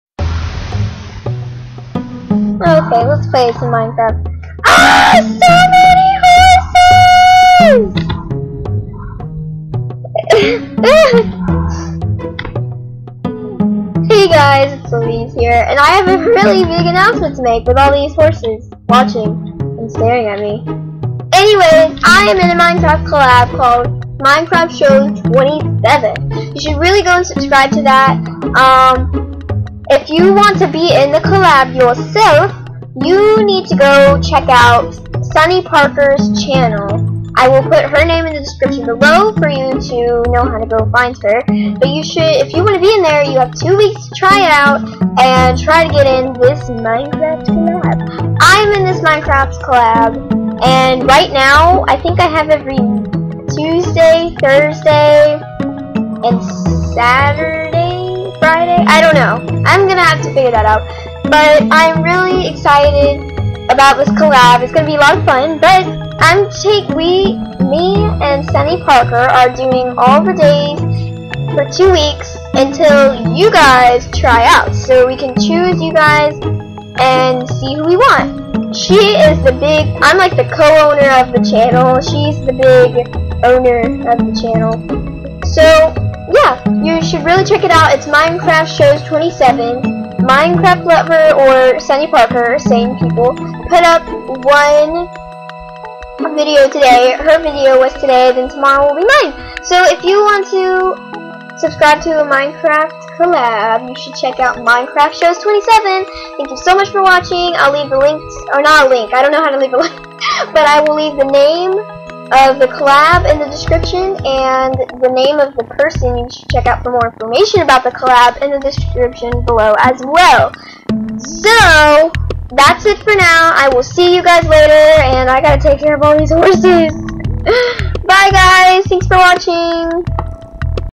I am in a collab!!!!
The collab is called MinecraftShows27. if you want to be in the collab to you can go to Sunny Parkers Channel! I hope you can get in! It is only minecraft! To know more about the collab you can watch Sunny Parkers i am starting a collab video!